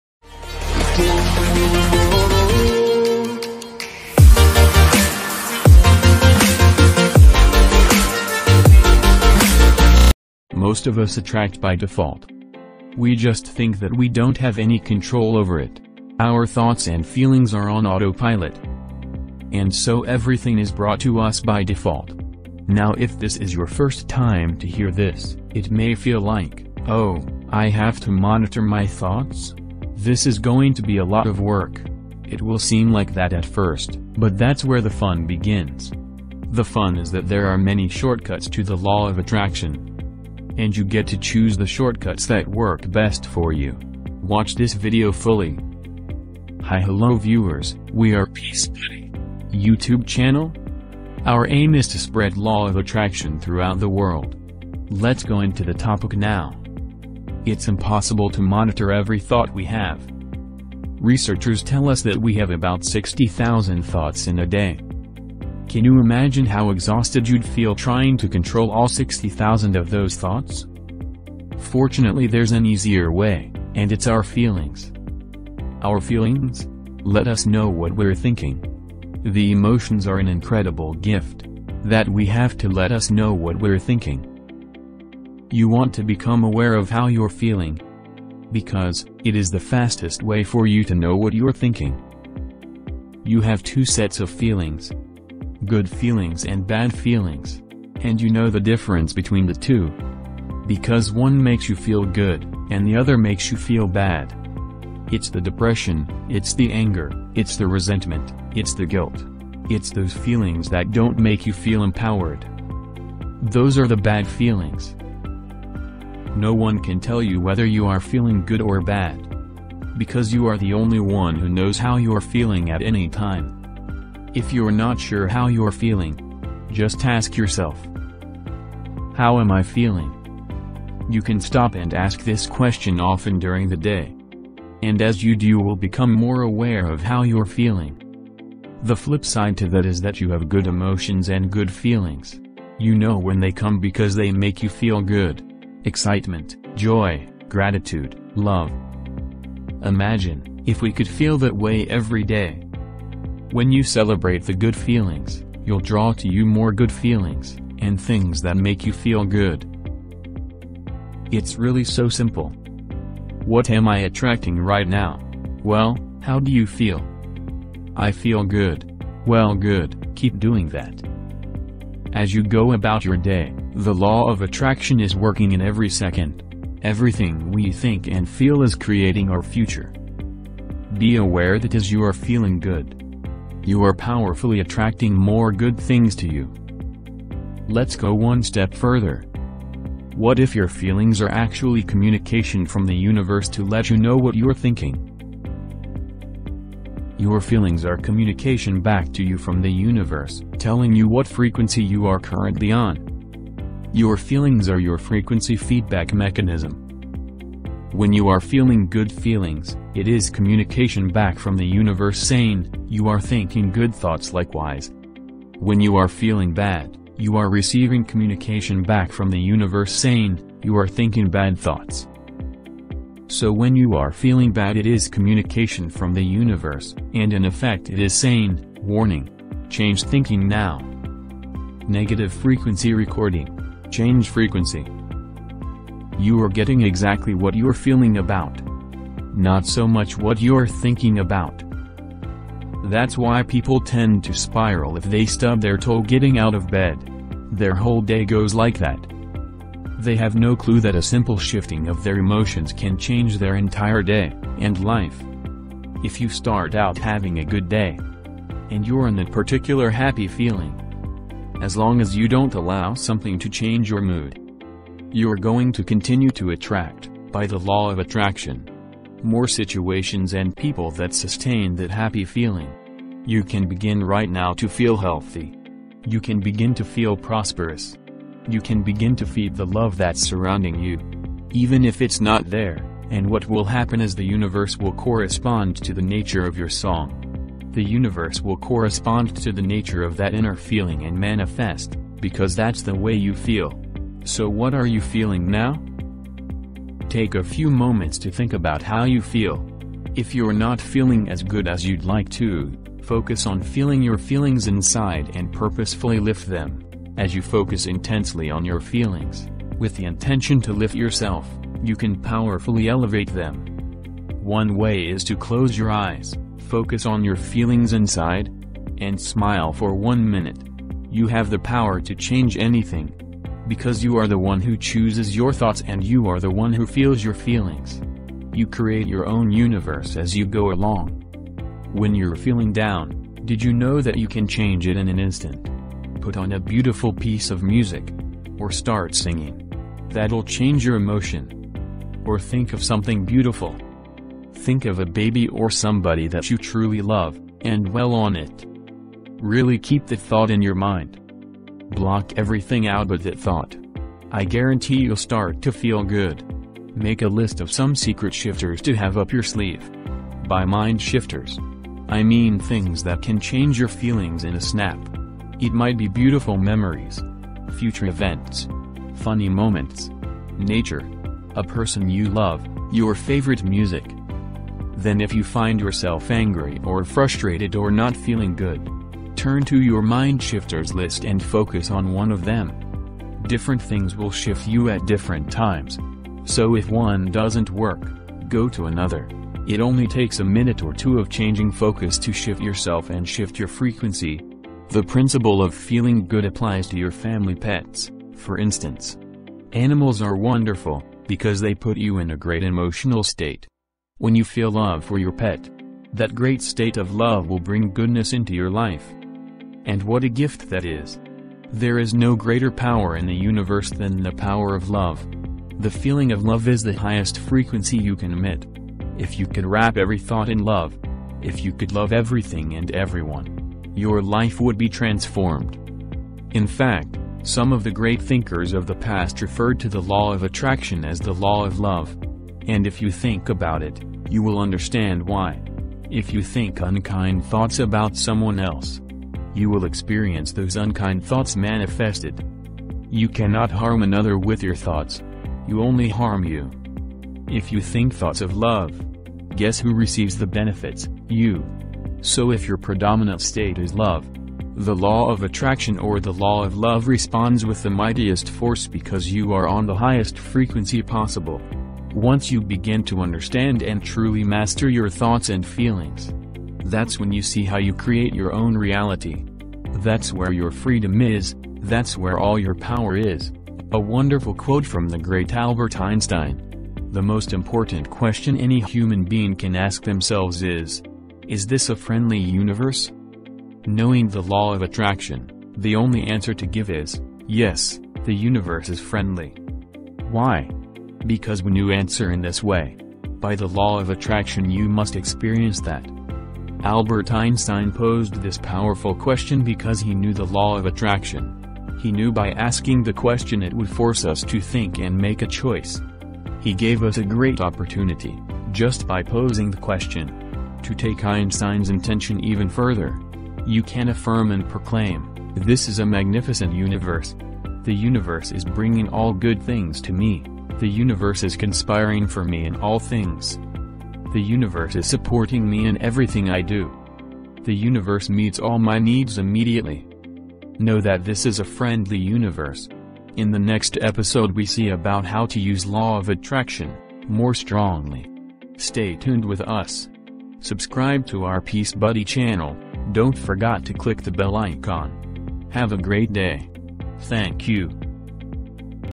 🔴 Law of attraction explained | the law of attraction episode 3 |{how to Manifest things faster?}
Peacebuddy creating content of law of attraction which helps you to achieve your dreams.(🔴 Law of attraction explained | the law of attraction episode 2 |{how to Manifest things faster?}). We use new tips and stratergies to achieve law of attraction in a short time.before watching this video watch previous video so only you can easily understand easily. Watch this video fully don’t skip. how should we manifest our thoughts was explained in this video.

Peace Buddy created the law of attraction series episode 3. in this episode we explained about how should we use our thoughts!. this video 100% useful for law of attraction or secret users and also useful for people who want to know about law of attraction.using the law of attraction you can change your life style.